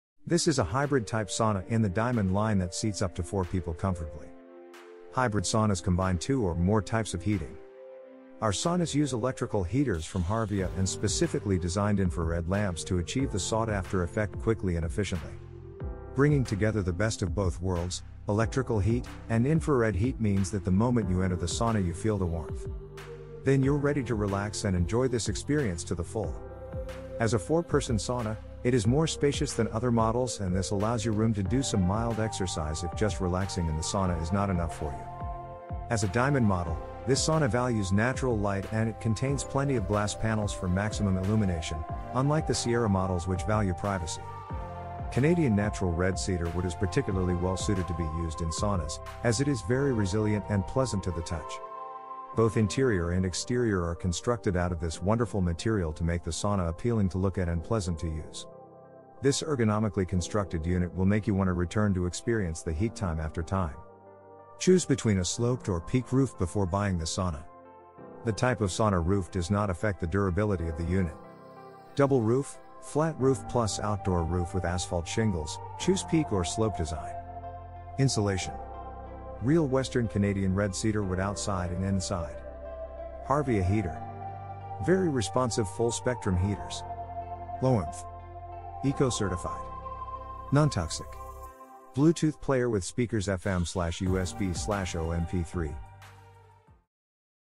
Diamond 4 Slope/Peak
This is a hybrid type sauna in the Diamond line that seats up to four people comfortably.

Hybrid saunas combine two or more types of heating.

Our saunas use electrical heaters from Harvia and specifically designed infra-red lamps to achieve the sought-after effect quickly and efficiently.

Bringing together the best of both worlds, electrical heat, and infra-red heat means that the moment you enter the sauna you feel the warmth.

Then you’re ready to relax and enjoy this experience to the full.

As a four-person sauna, it is more spacious than other models and this allows you room to do some mild exercise if just relaxing in the sauna is not enough for you.

As a Diamond model, this sauna values natural light and it contains plenty of glass panels for maximum illumination, unlike the Sierra models which value privacy.

Canadian natural red cedar wood is particularly well-suited to be used in saunas, as it is very resilient and pleasant to the touch.

Both interior and exterior are constructed out of this wonderful material to make the sauna appealing to look at and pleasant to use.

This ergonomically constructed unit will make you want to return to experience the heat time after time.

Choose between a sloped or peak roof before buying this sauna. The type of sauna roof does not affect the durability of the unit.

Double Roof ( Flat Roof + Outdoor Roof With Asphalt Shingles, Choose Peak Or Slope Design)
Insulation
Real Western Canadian Red Cedar Wood Outside And Inside
Harvia Heater
Very Responsive Full Spectrum Heaters
Low Emf
Eco-Certified
Non-Toxic
Bluetooth Player With Speakers FM/USB/AUX/MP3

INTERIOR SPECIFICATION
Width: 67.5"
Depth: 57.5"
Height: 73"
Bench: 65.5" x 22"

EXTERIOR SPECIFICATION
Width: 71.5"
Depth: 61.5"
Height: 96"

ROOF SPECIFICATION
Roof Width: 81.5"
Roof Depth: 71.5"

UNIT WEIGHT
Number of Boxes: 7
Product Weight: 1050 lbs
Shipping Weight: 1100 lbs

FEATURES
Adjust. Temp.: 0-189 F
Heaters Number: 11 Heaters
Heaters Type: Electrical Heater + Infrared Heaters
Timer: 0 - 60 mins
Near/Mid IR Wavelength: 0 to 5 microns
Far IR Wavelength: 6 to 12 microns

POWER USAGE/AMPS
Amperage: 20 Amp - 2 separate designated circuits, 1 per each heating technology
Plug: NEMA 6-20p
Voltage: 220/240 V
Wattage: 3750 W
Electrical Heater Wattage: 4500 W